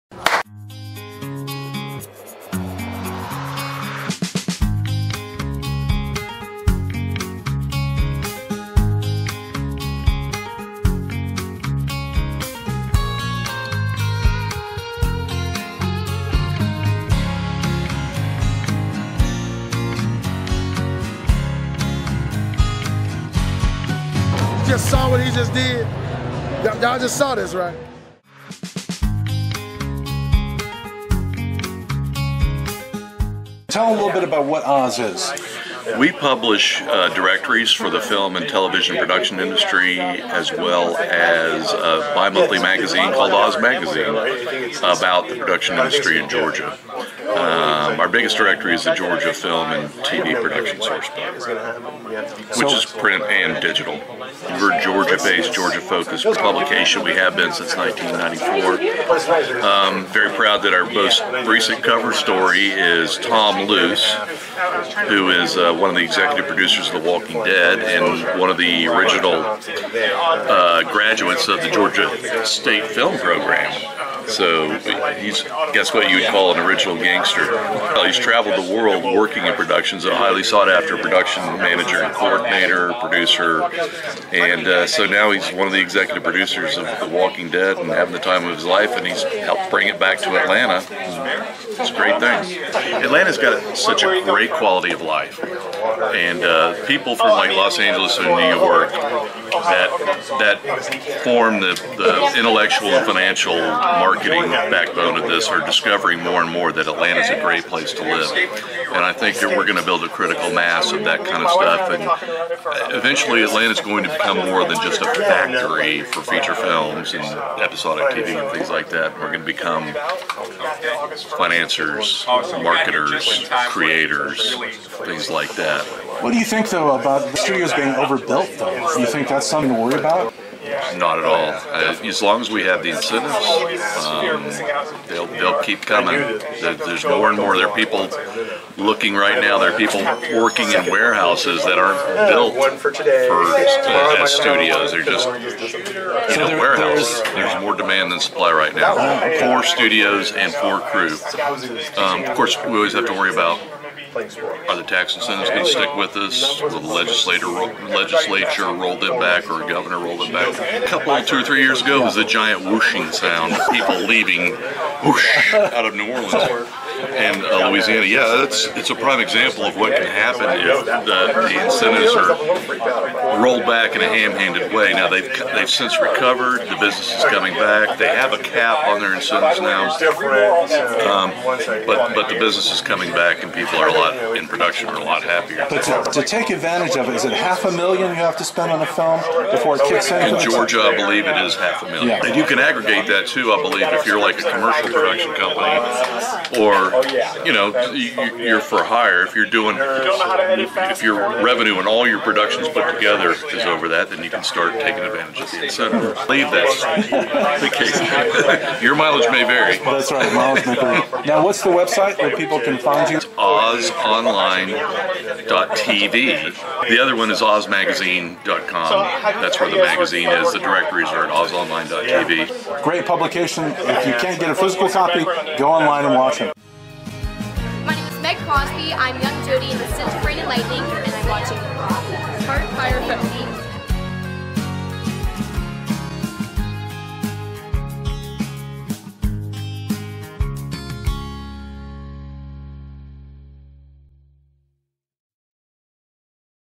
GP Studios Open House: Gary Powell - Oz Magazine Publisher/Editor-in-Chief
Conversation with Gary Powell,Oz Magazine Publisher/Editor-in-Chief. We spoke during GP Studios Open House. We discuss Oz Magazine - The Georgia Film Industry Resource, Ozonline.tv ,Tom Luse – Executive Producer of 'The Walking Dead' and a bit more.

GP Studios Open House: Creative Director Walter Biscardi Jr.

Oz Interview with Tom Luse - Above the Line with Tom Luse: Light from the Dead.